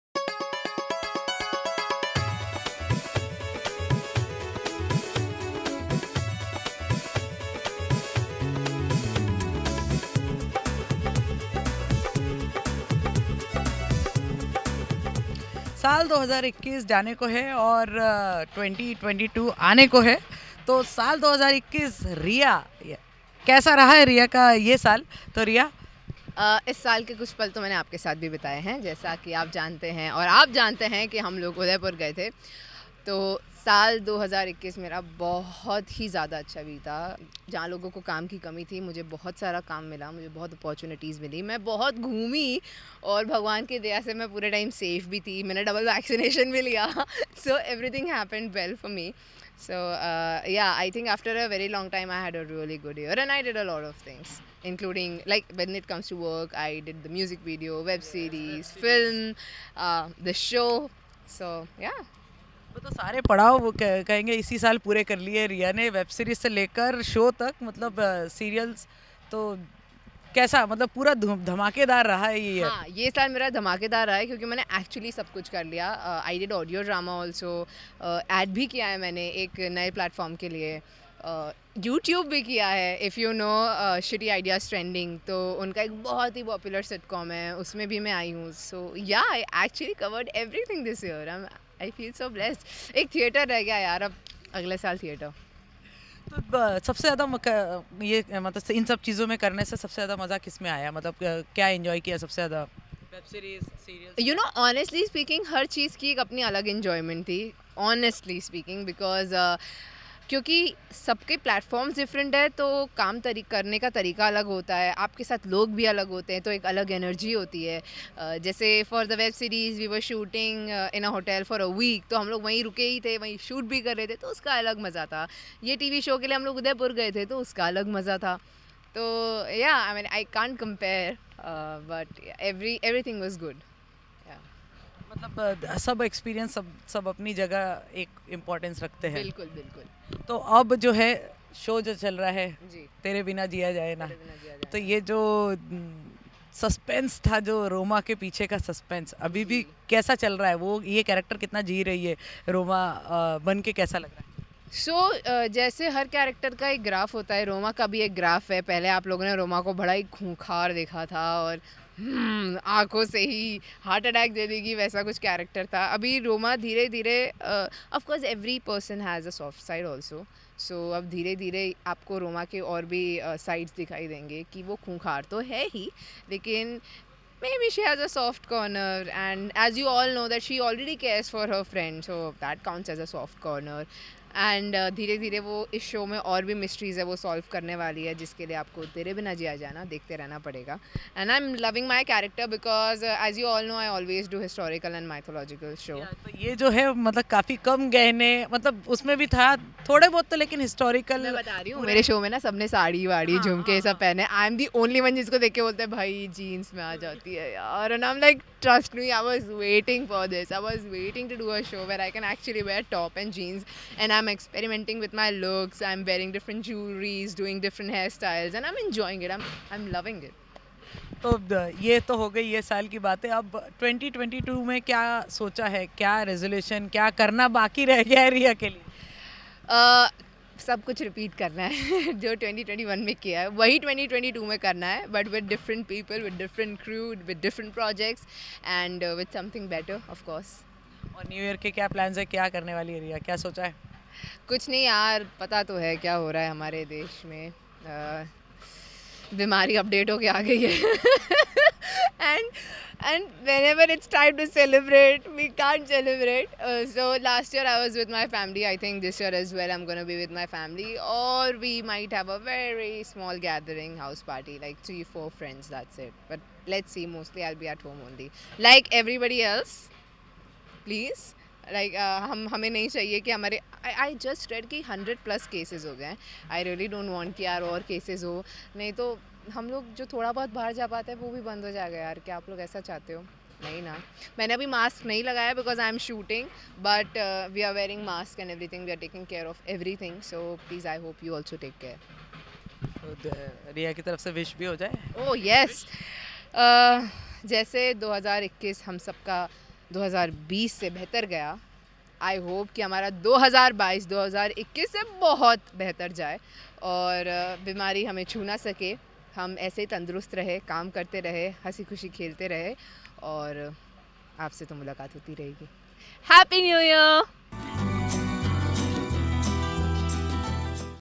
Recap Of Riya Deepsi's 2021!
An interview with Riya Deepsi were she shares about her 2021 journey..